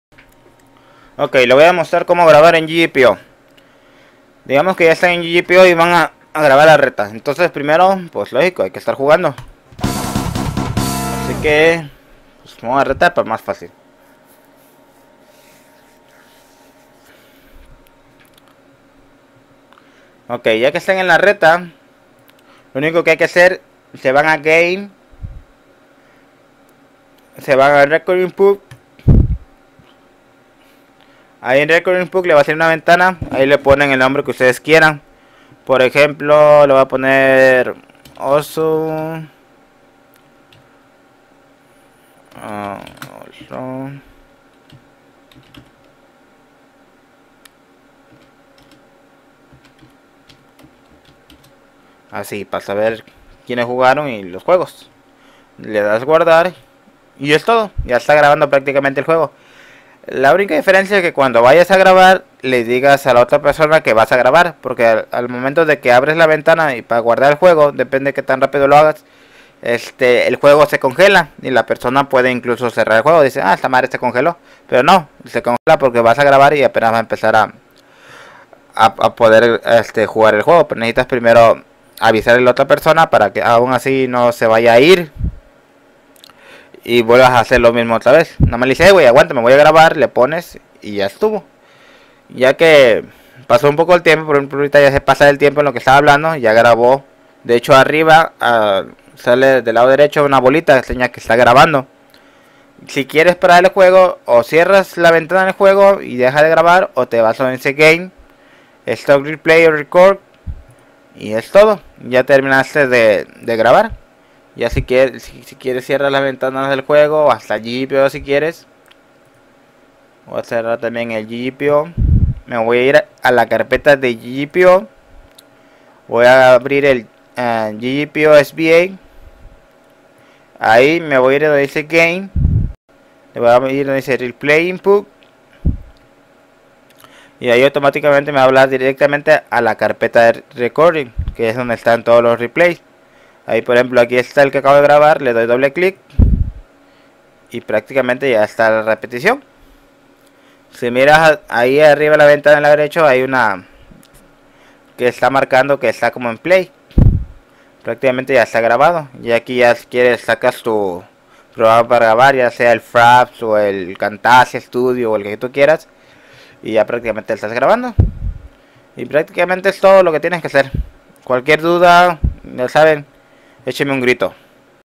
Grabando en GGPO1080p
breve tutorial como grabar los juegos en ggpo.